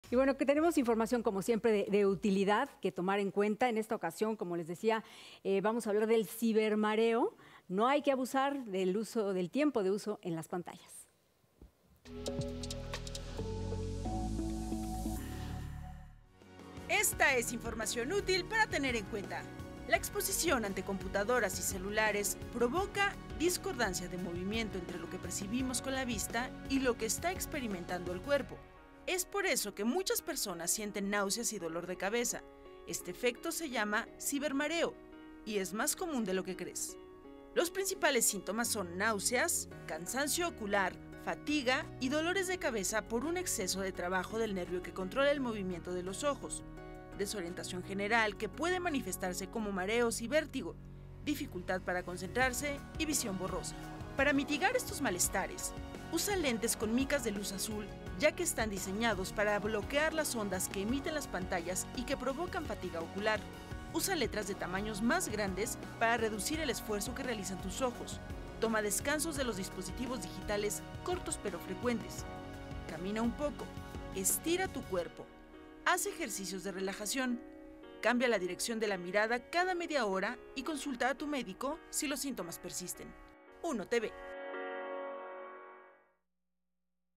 Cibermareo, un efecto del uso de pantallas que no debes minimizar y que puedes controlar
Si de repente sientes náuseas y el dolor de cabeza es recurrente, puede tratarse de cibermareo, una afectación provocada por el uso excesivo de pantallas y que puedes controlar con algo de disciplina y algunas sencillas acciones...